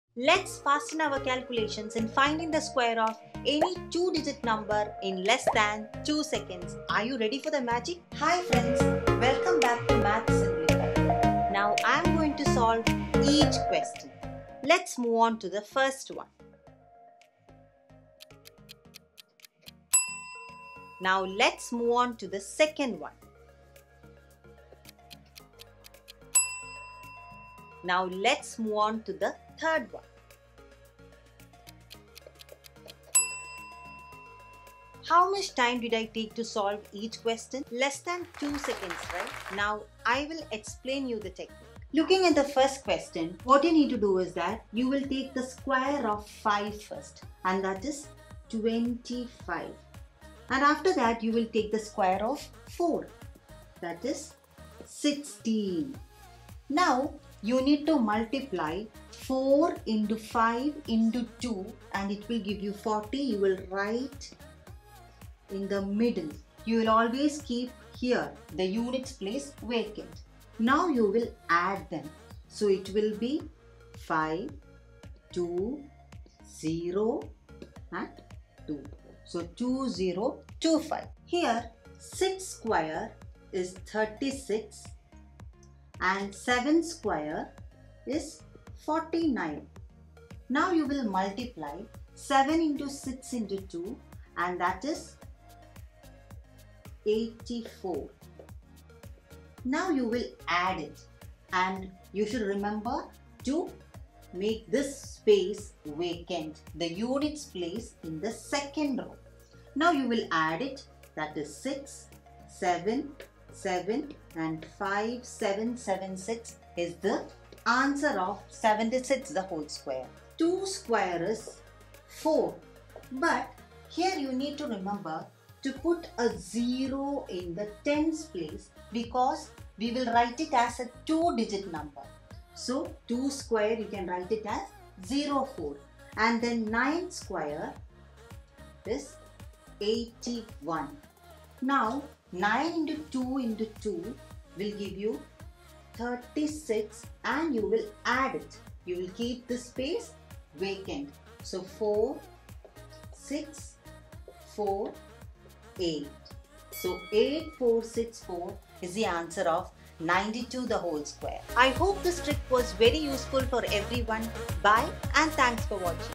SQUARE OF 2 DIGIT NUMBER TRICK | LESS THAN 2 SECONDS | VEDIC MATHS TRICK
You can find the square of any 2-digit number in less than 2 seconds by using this Amazing trick.

Don’t forget to suggest my channel to someone who needs it!